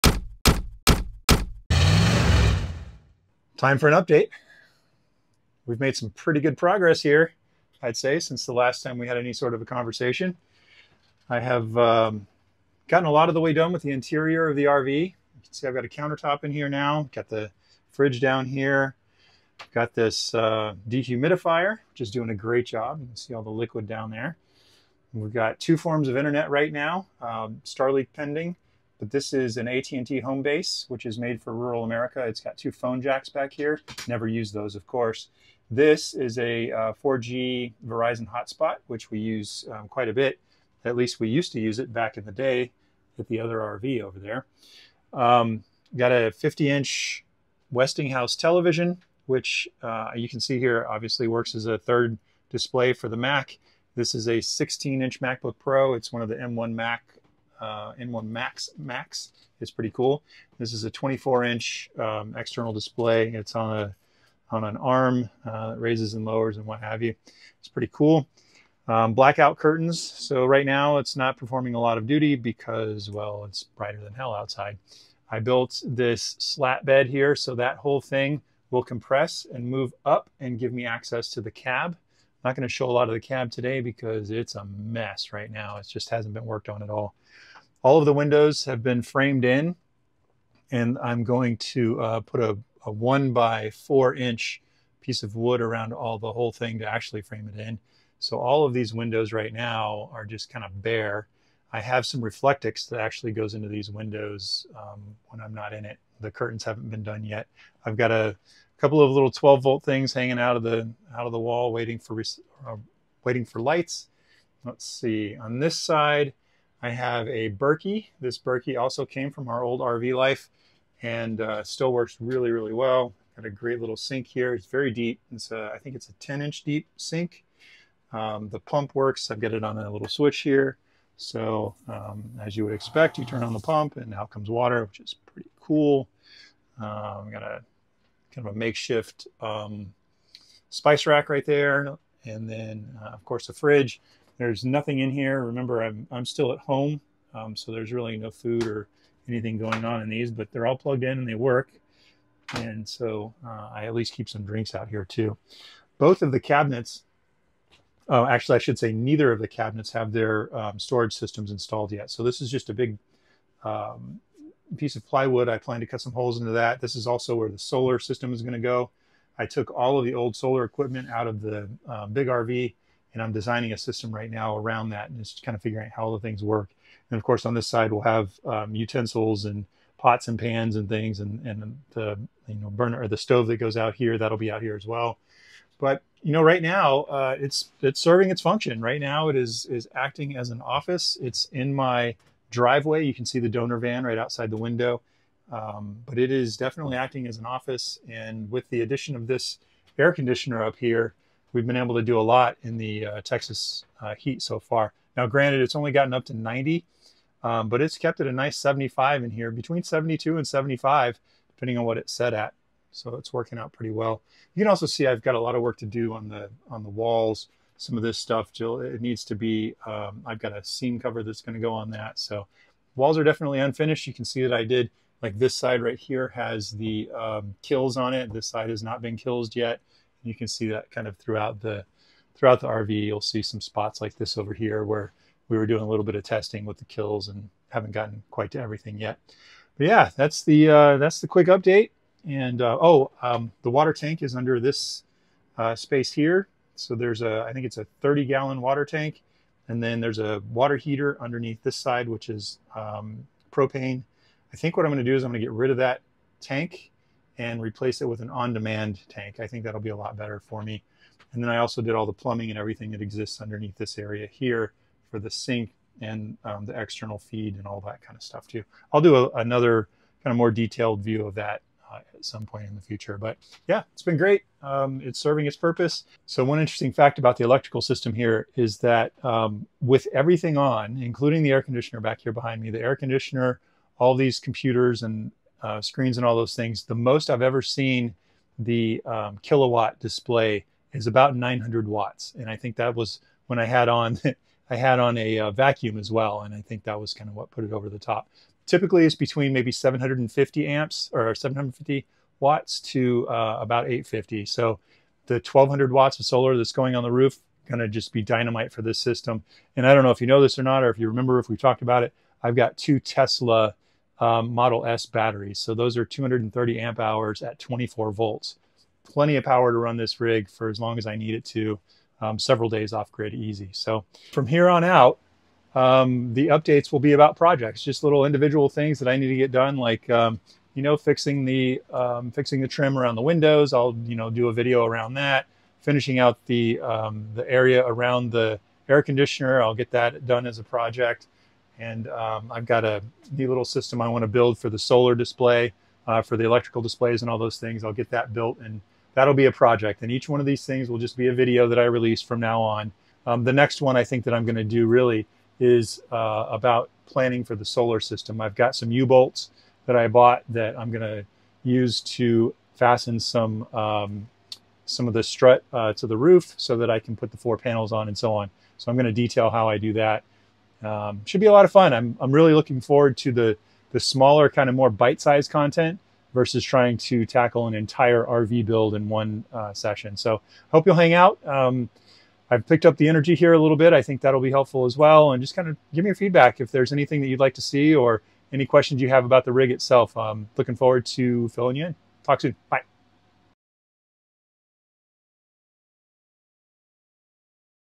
This Classic RV is also a mobile office!
This is a quick update video on the RV project. It's a 20 foot 1981 Coachmen Surrey Class C RV that I've gutted to the studs and have been rebuilding. It's reached a milestone in that I'm using it day to day as my office!

The video shows a few features like the 50" 4K TV, the computer setup, and several others.

It's come a long, long ways! Hope you enjoy it!

Camera: iPhone 14 Pro Max (using Continuity Camera on macOS Ventura), and an iPhone 13 Pro Max for the rest of the video.
Microphone: Hollyland LC1
Edited on an Apple 16" MacBook Pro with M1 Max